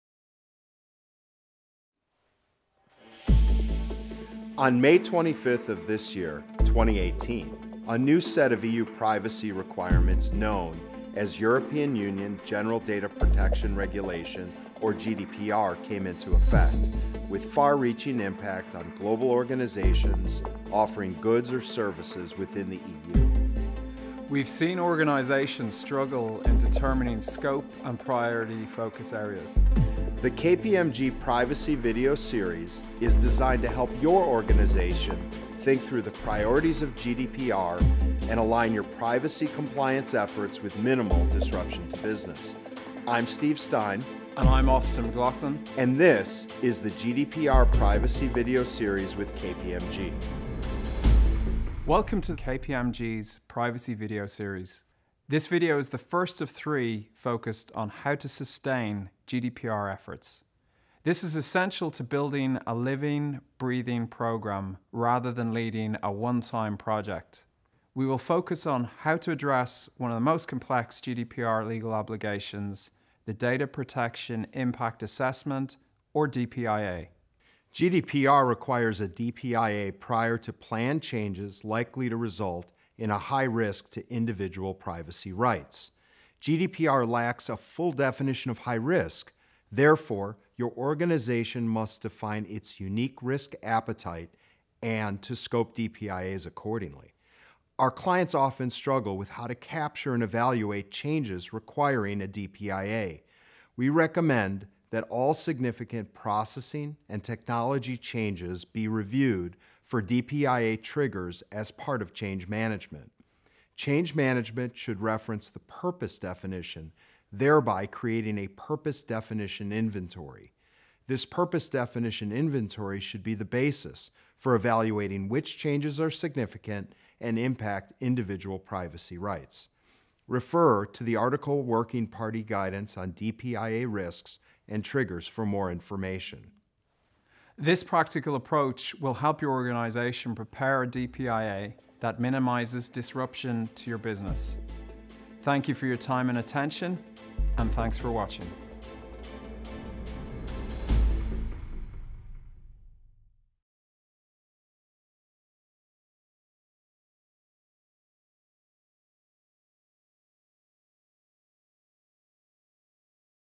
GDPR How to Sustain Efforts: Privacy Impact Assessment (PIA) and Purpose Definition
This GDPR video is the first of three that will discuss building a living long-lasting, sustainable GDPR program. It will cover GDPR legal obligations, which is the Data Protection Impact Assessment or DPIA.

 KPMG.com/US/GDPR